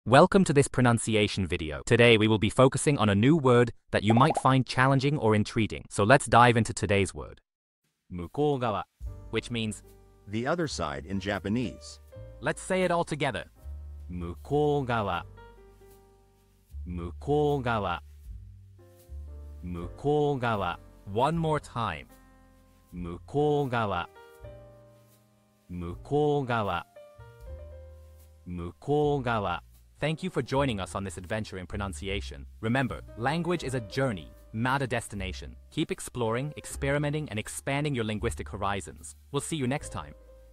How to pronounce mukougawa | 向こう側 (the other side in Japanese)
Master the Pronunciation of 'mukougawa | 向こう側' - which means : the other side in Japanese 📝 with @PronunciationJapanese 🎓🗣️ - [Your Guide to Japanese]

Learn to pronounce 'mukougawa' perfectly in Japanese with our easy and engaging tutorial by @PronunciationJapanese! 📖📽️ In this comprehensive video, we provide you with native speaker examples 🗣️👂 and phonetic breakdowns 🎵 to ensure you get the pronunciation just right.